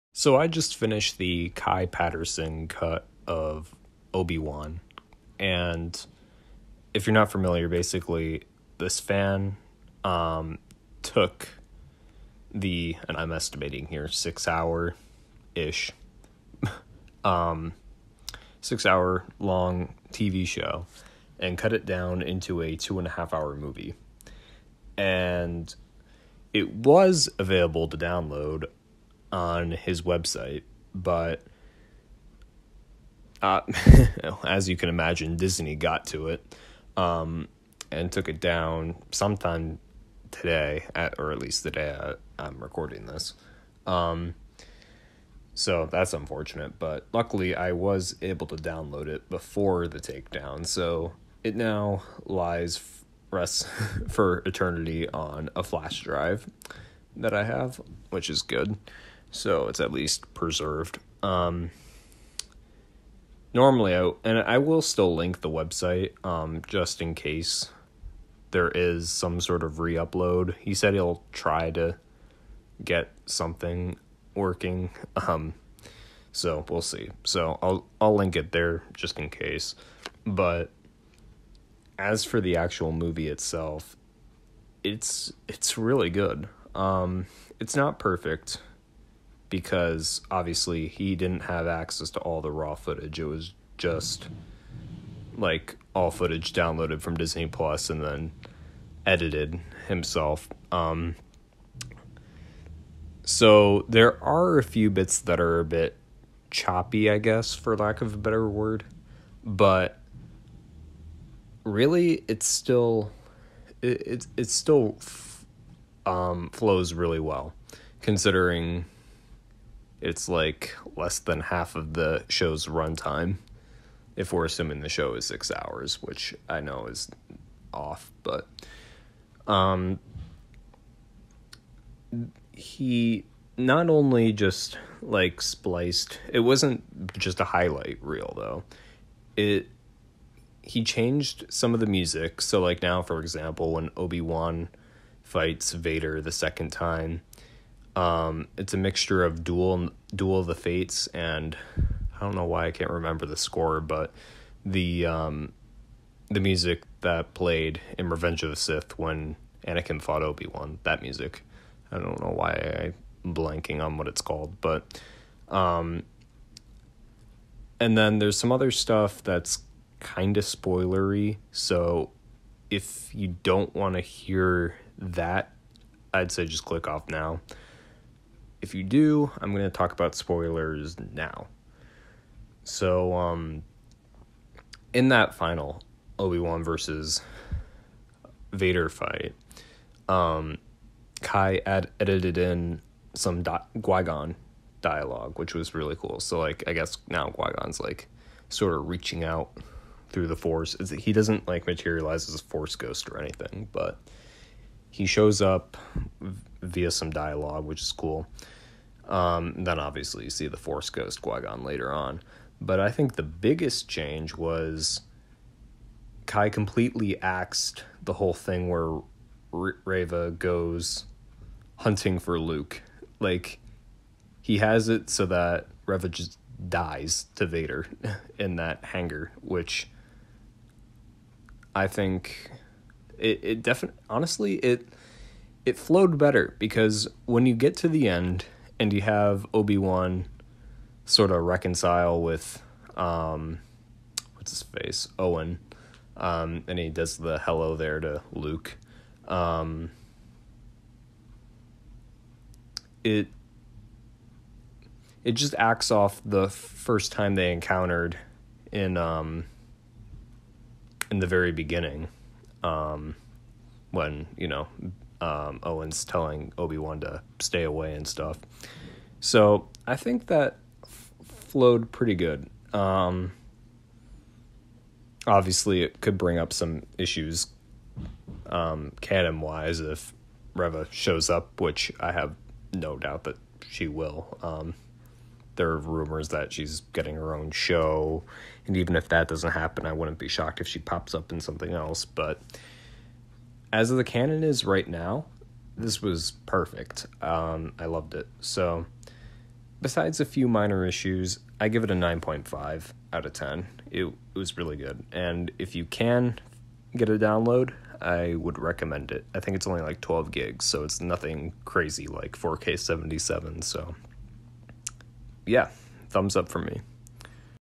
Kenobi Fan Edit Review
This is a review of the fan made Kenobi project. It takes the tv series and slims it into a 2.5 hour movie.